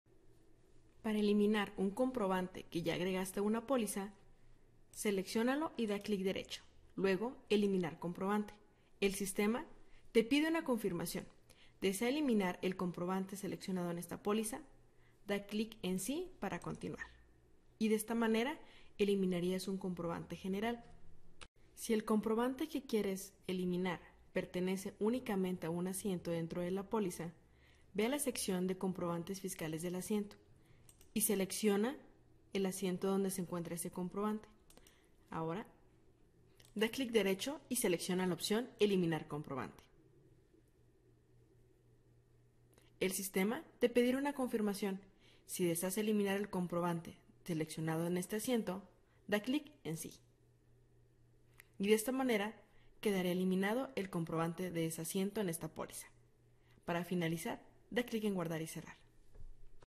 Microsip Contabilidad: Cómo eliminar comprobantes de una póliza
Microsip es el ERP que se adapta a tu empresa.